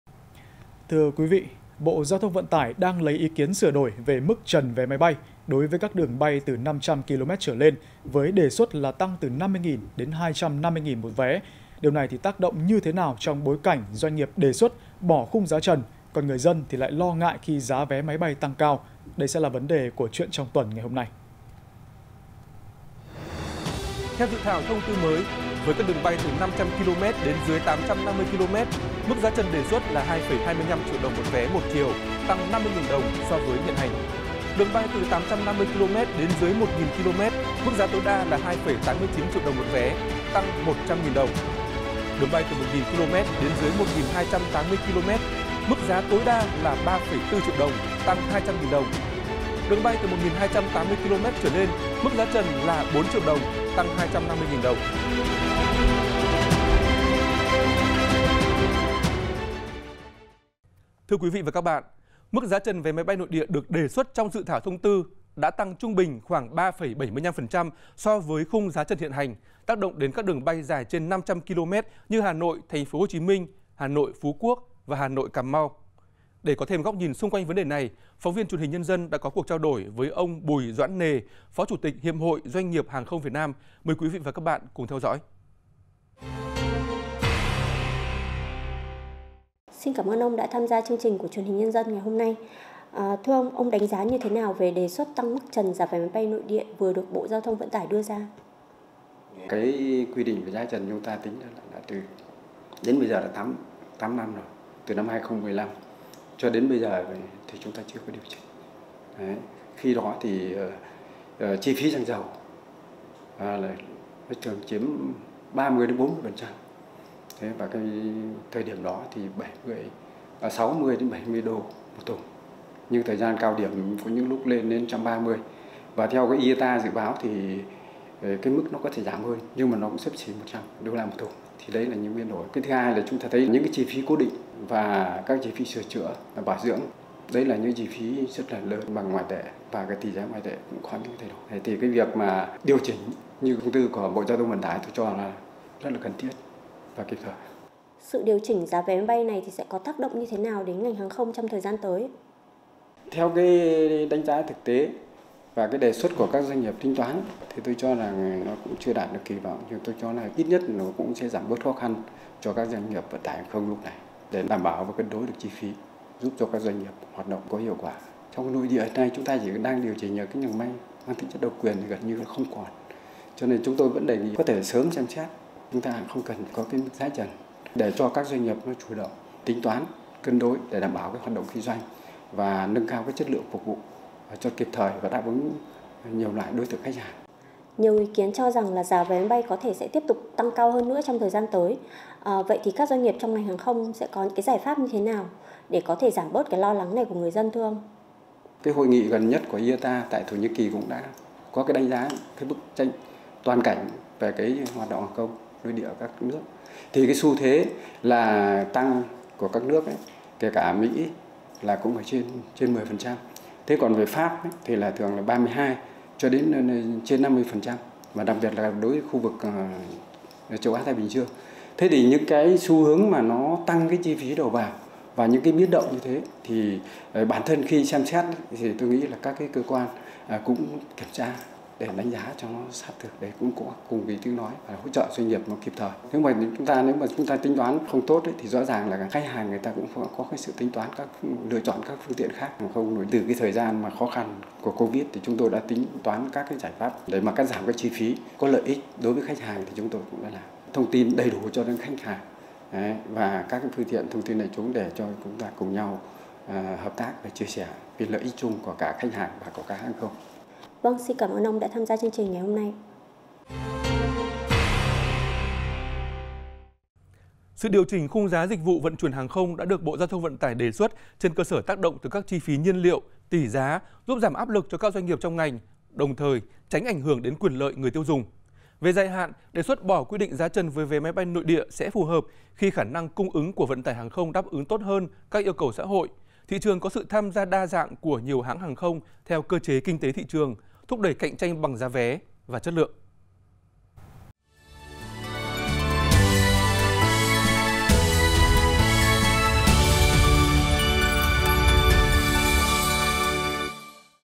Xung quanh đề xuất tăng giá trần vé máy bay nội địa
Xung quanh đề xuất tăng giá trần vé máy bay nội địa | Truyền Hình Nhân Dân
1. Dự Báo Thời Tiết Hôm Nay